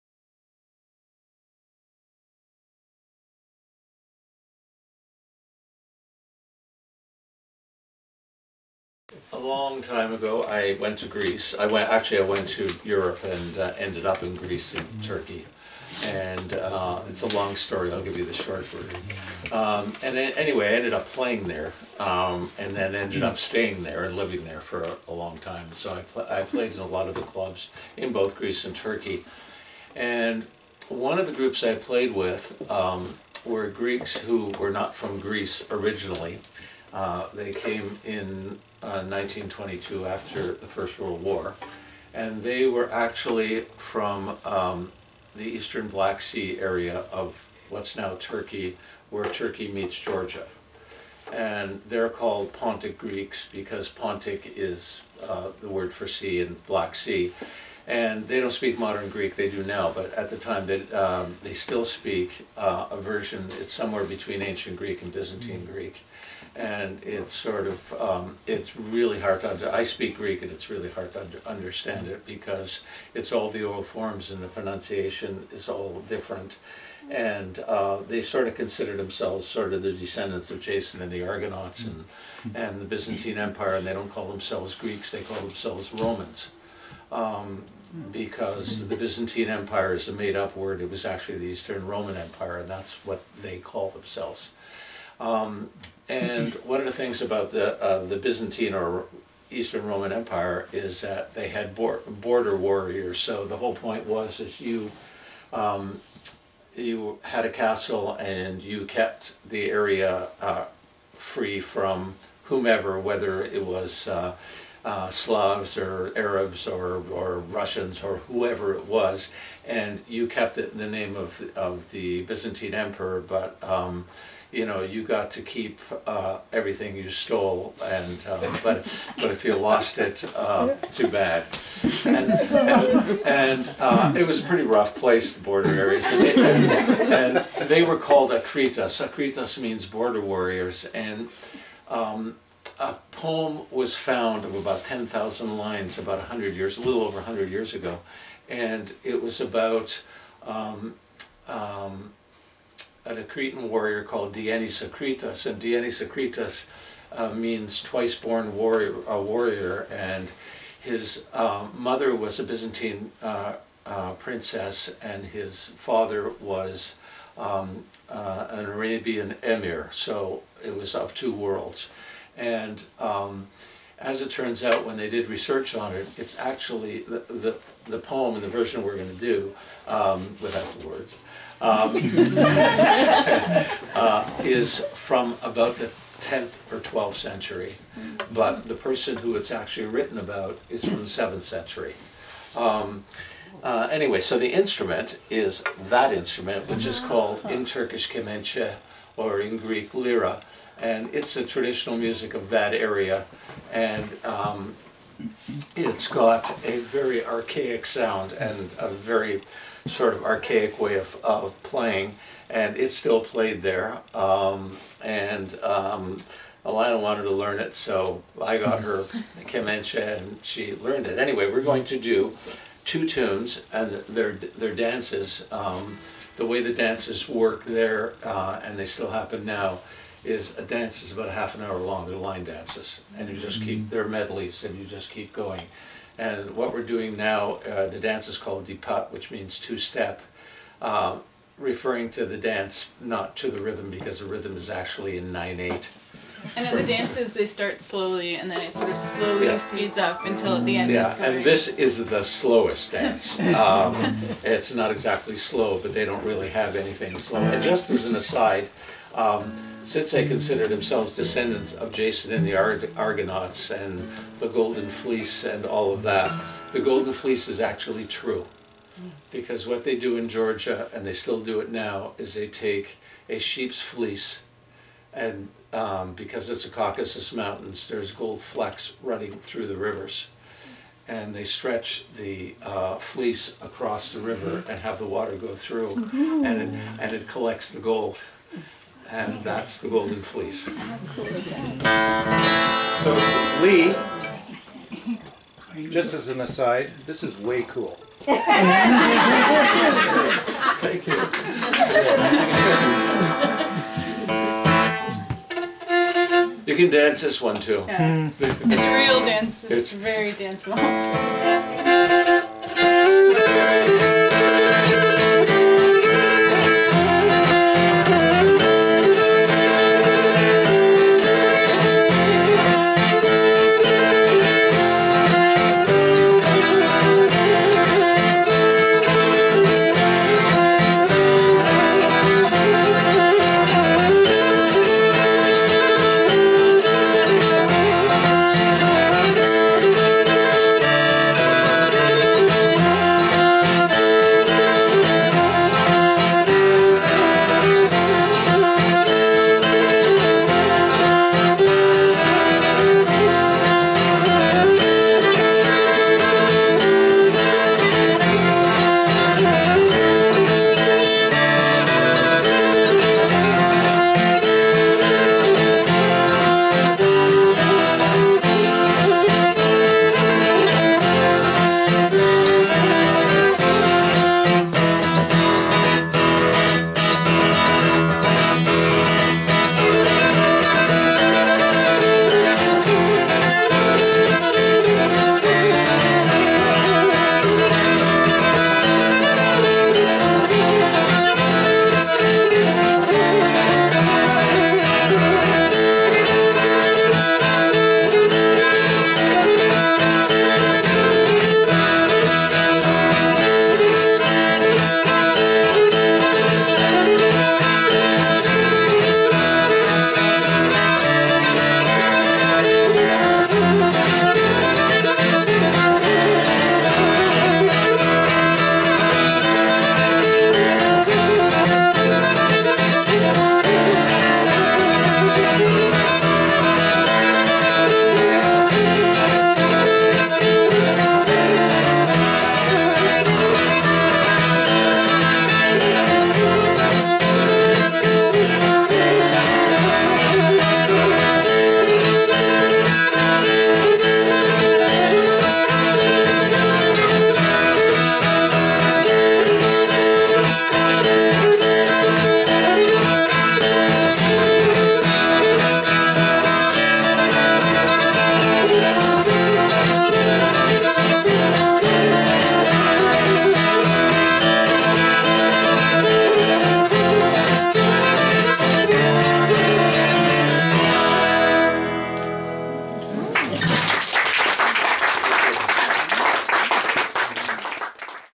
Alana & Leigh Cline - Pontic Music - House Concert 2017
House Concert April 2017

Talk and performance of 2 Pontic Greek dance songs